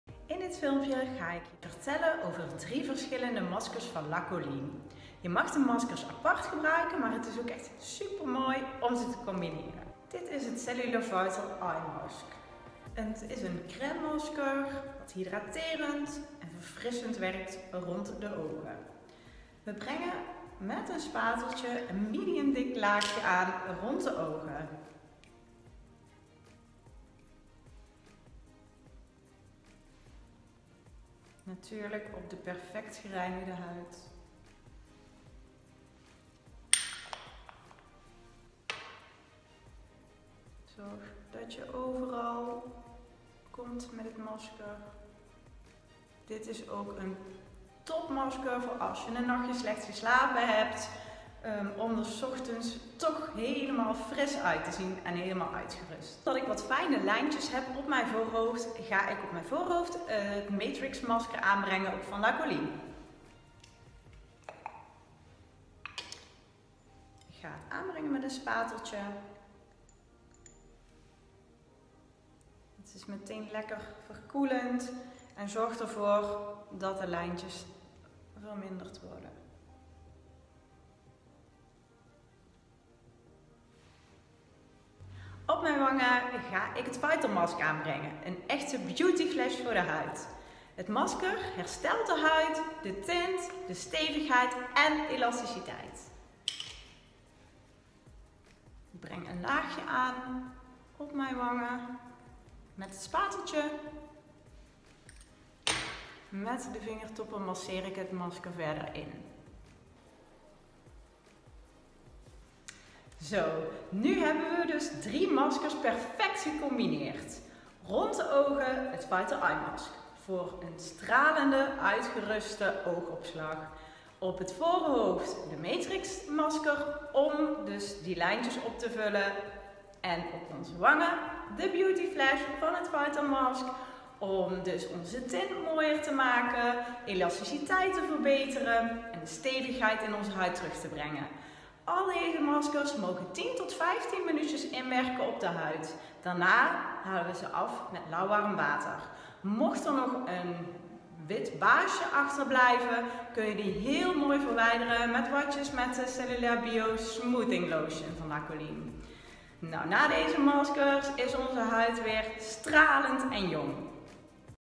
La Colline Multi-Mask laat zien dat je meer dan 1 masker tegelijk kan gebruiken!
Wist je dat je door meerdere maskers aan te brengen heel specifiek kan werken aan ieder ‘probleem’ gebied?
We noemen dit een multi mask. Verschillende maskers op verschillende zones van het gezicht.
Jessie gebruikt voor rondom haar ogen het La Colline Vital Eye mask.
De smeuïge, zachte textuur in combinatie met doorbloeding stimulerende (korenbloem) en kalmerende (roos) plantenextracten maakt de blik onmiddellijk glad en fris. Kringen worden direct verminderd.
Op het voorhoofd kiest Jessie voor het heerlijk gezichtsmasker an La Colline Matrix die met het R3-complex zorgt voor een zachte, stralende huid. Het masker gaat de fijne lijntjes tegen, met als resultaat een gladde huid met een frisse uitstraling. Voor direct resultaat!
De wangen worden verwend met La Colline Cellular Vital Mask. Dit anti-aging hydraterende masker geeft direct een boost aan de uitstraling van het gezicht. Het stimuleert de zuurstoftoevoer naar de huidcellen en geeft een actieve hydratatie. De huid is verzacht en hersteld en voelt heerlijk zacht met een jeugdige elasticiteit.
10 tot 15 minuten laten intrekken en de huid is weer helemaal stralend met direct zichtbaar resultaat.

La Colline Cellular Vital Eye Mask
Crème oogmasker, werkt direct hydraterend, verzachtend en geeft glans. Onmiddellijk verhelderend effect op donkere kringen.
Hydraterend, vochtafdrijvend en verzacht de oogcontouren
Kalmerend en direct revitaliserend op de oogcontouren en een direct gevoel van frisheid

La Colline Matrix mask is een masker dat is gericht op het reduceren van rimpels en het verstevigen van de huid. Hij verfrist de huid, verstevigd de huid en reduceert rimpels. ook heeft hij een directe herstelling van de huid

Wil je anti-aging producten bestellen, zodat je je huid thuis natuurlijk jonger maakt?

Breitnerstraat 2A
Eindhoven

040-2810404